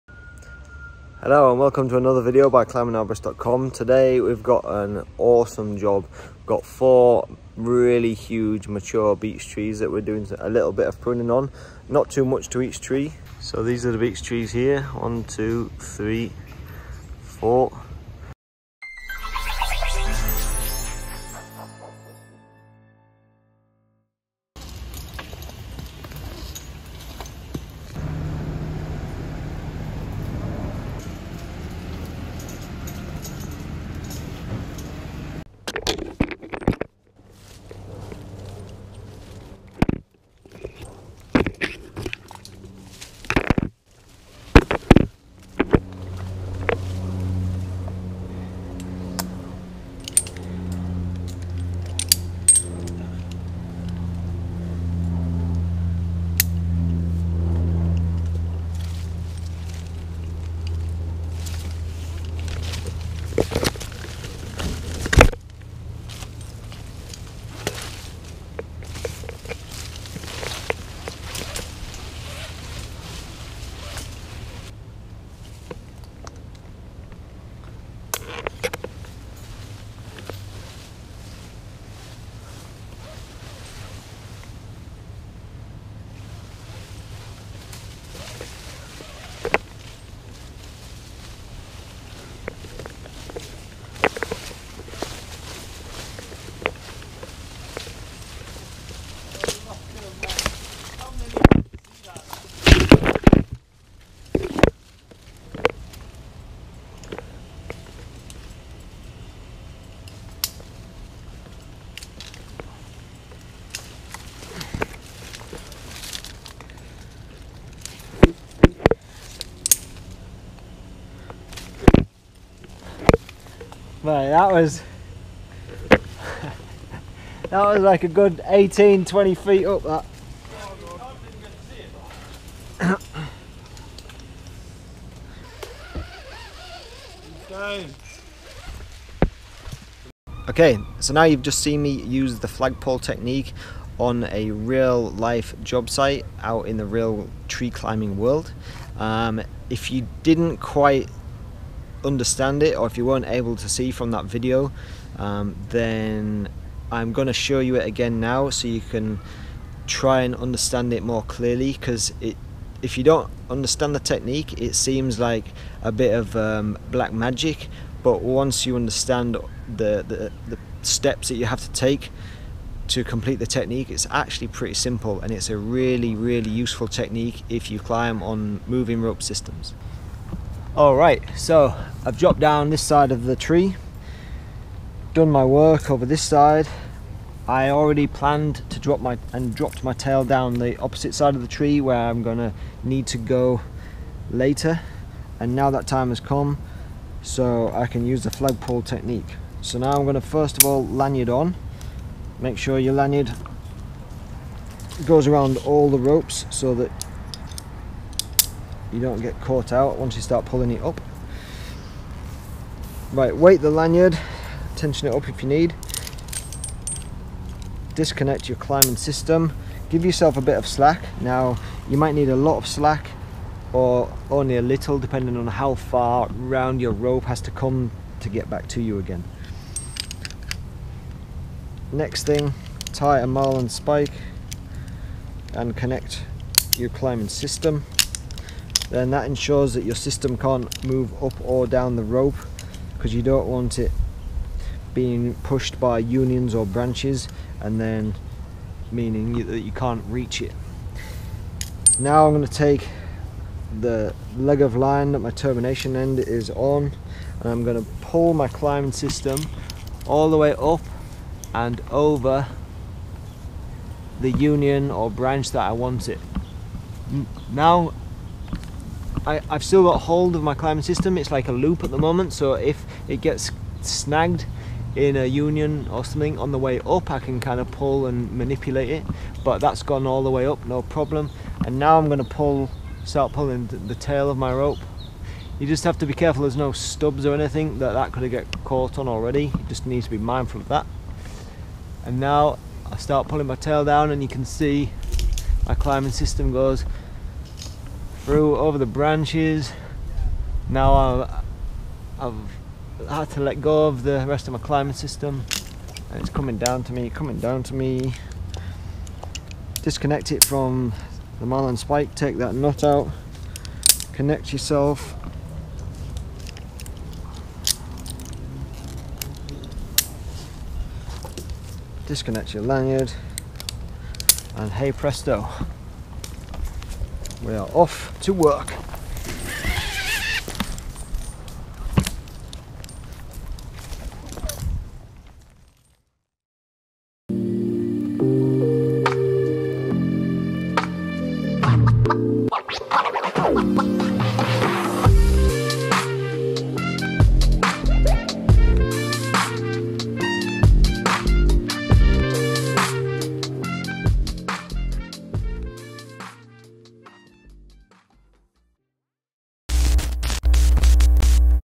Real world use of the Flagpole technique / Ponytail redirect in tree work
Dan shows how useful the flagpole technique is when climbing on a moving rope system (M.R.S) , saving time and energy. Dan the walks through the system step by step so you can see clearly, and understand it is actually quite simple when you are familiar with this technique.
Practice this a few times on the ground until you understand it, then put this into your bag of tricks. It will bring a big smile to your face when you get the right opportunity to use it.

to find your next job